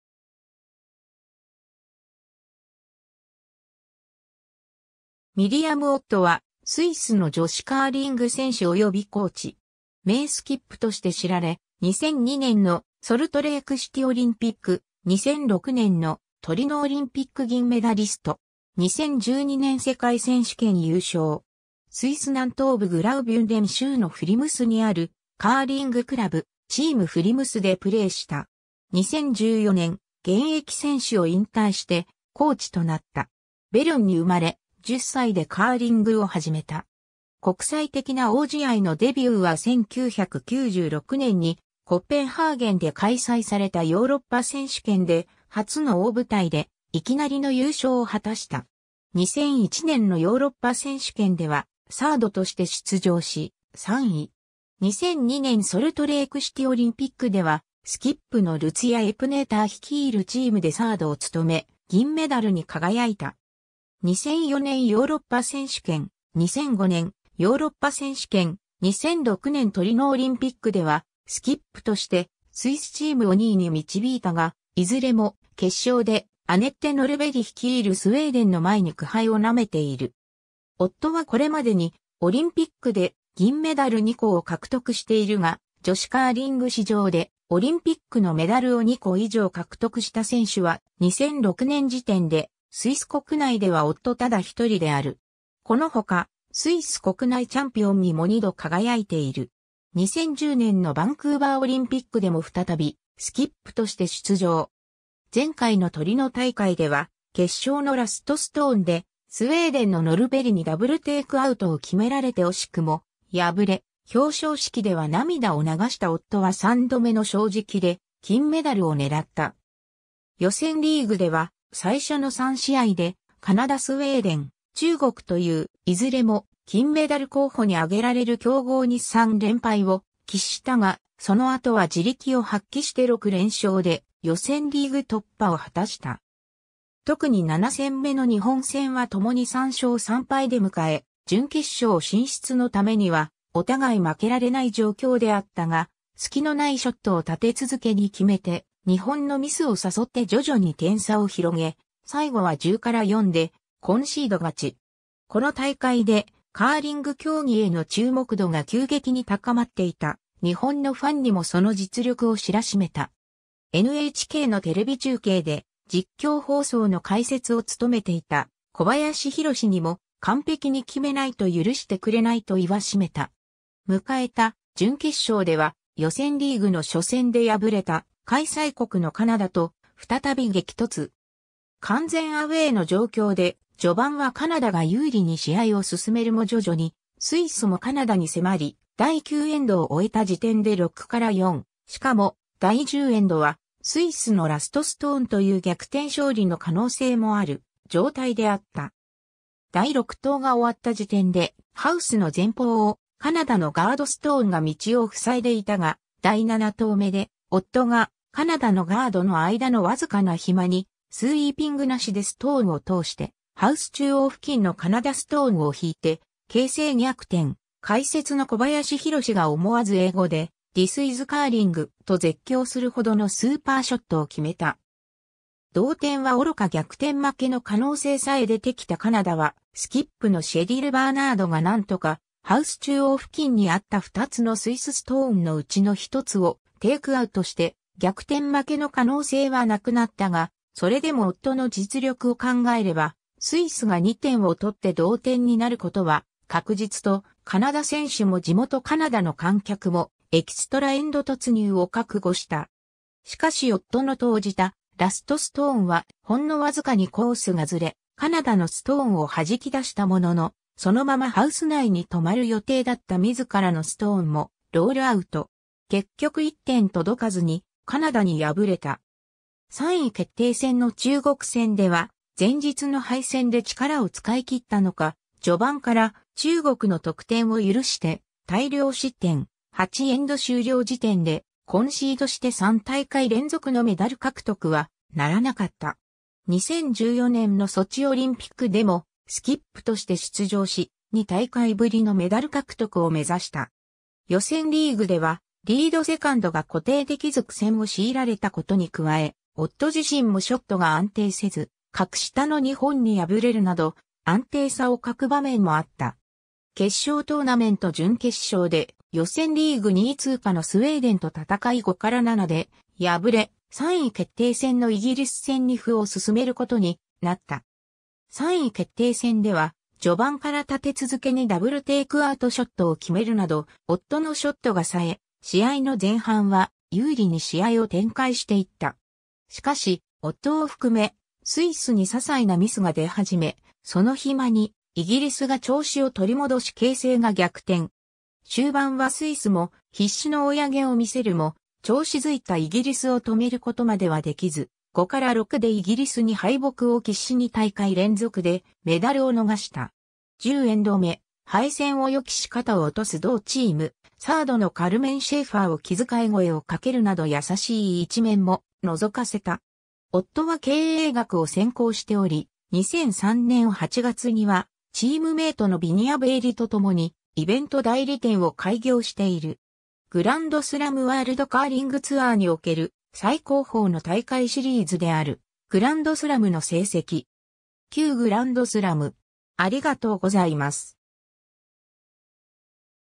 ミリアム・オット
ミリアム・オット（Mirjam Ott、1972年1月27日 - ）は、スイスの女子カーリング選手およびコーチ。
名スキップとして知られ、2002年のソルトレイクシティオリンピック、2006年のトリノオリンピック銀メダリスト。
2012年世界選手権優勝。
スイス南東部グラウビュンデン州のフリムスにあるカーリングクラブ、「チーム・フリムス」(TeamFlims) でプレーした。
2014年、現役選手を引退してコーチとなった。
 ベルンに生まれ、10歳でカーリングをはじめた。
国際的な大試合のデビューは1996年にコペンハーゲンで開催されたヨーロッパ選手権で、初の大舞台でいきなりの優勝を果たした。
2001年のヨーロッパ選手権（フィンランド、ヴィエルメキ）ではサードとして出場し、3位。
2002年ソルトレークシティオリンピックではスキップのルツィア・エプネーター率いるチームでサードを務め、銀メダルに輝いた。
 2004年ヨーロッパ選手権（ブルガリア、ソフィア）、2005年ヨーロッパ選手権（ドイツ、ガルミッシュ＝パルテンキルヒェン）、2006年トリノオリンピックではスキップとしてスイスチームを2位に導いたが、いずれも決勝で、アネッテ・ノルベリ率いるスウェーデンの前に苦杯を舐めている。
オットはこれまでにオリンピックで銀メダル2個を獲得しているが、女子カーリング史上でオリンピックのメダルを2個以上獲得した選手は、2006年時点でスイス国内ではオットただ一人である。
このほか、スイス国内チャンピオンにも二度（1997年、2004年）輝いている。
 2010年のバンクーバーオリンピックでも再びスキップとして出場。
前回のトリノ大会では決勝のラストストーンでスウェーデンのノルベリにダブルテイクアウトを決められて惜しくも破れ、表彰式では涙を流したオットは三度目の正直で金メダルを狙った。
予選リーグでは最初の3試合でカナダ・スウェーデン・中国といういずれも金メダル候補に挙げられる強豪に3連敗を喫したが、その後は地力を発揮して6連勝で予選リーグ突破を果たした。
特に7戦目の日本戦は共に3勝3敗で迎え、準決勝進出のためにはお互い負けられない状況であったが、スキのないショットを立て続けに決めて日本のミスを誘って徐々に点差を広げ、最後は10-4でコンシード勝ち。
この大会でカーリング競技への注目度が急激に高まっていた日本のファンにもその実力を知らしめた。
NHKのテレビ中継で実況放送の解説を務めていた小林宏にも「（日本側が）完璧に決めないと許してくれない（得点させてもらえない）」と言わしめた。
迎えた準決勝では予選リーグの初戦で敗れた開催国のカナダと再び激突。
完全アウェーの状況で序盤はカナダが有利に試合を進めるも徐々にスイスもカナダに迫り、第9エンドを終えた時点で6-4、しかも第10エンドはスイスのラストストーンという逆転勝利の可能性もある状態であった。
第6投が終わった時点でハウスの前方をカナダのガードストーンが道を塞いでいたが、第7投目でオットがカナダのガードの間のわずかな隙にスウィーピングなしでストーンを通してハウス中央付近のカナダストーンを弾いて形勢逆転、解説の小林宏が思わず英語で"This is curling!"と絶叫するほどのスーパーショットを決めた。
同点はおろか逆転負けの可能性さえ出てきたカナダはスキップのシェリル・バーナードがなんとかハウス中央付近にあった2つのスイスストーンのうちの1つをテイクアウトして逆転負けの可能性はなくなったが、それでもオットの実力を考えればスイスが2点をとって同点になることは確実とカナダ選手も地元カナダの観客もエキストラエンド（延長戦）突入を覚悟した。
しかしオットの投じたラストストーンはほんの僅かにコースがずれ、カナダのストーンをはじき出したものの、そのままハウス内に留まる予定だった自らのストーンもロールアウト。
結局1点とどかずにカナダに敗れた。
3位決定戦の中国戦では前日の敗戦で力を使い切ったのか、序盤から中国の得点を許して大量失点、8エンド終了時点でコンシードして3大会連続のメダル獲得はならなかった。
 2014年のソチオリンピックでもスキップとして出場し、2大会ぶりのメダル獲得を目指した。
予選リーグでは、リード・セカンドが固定できず苦戦を強いられたことに加え、...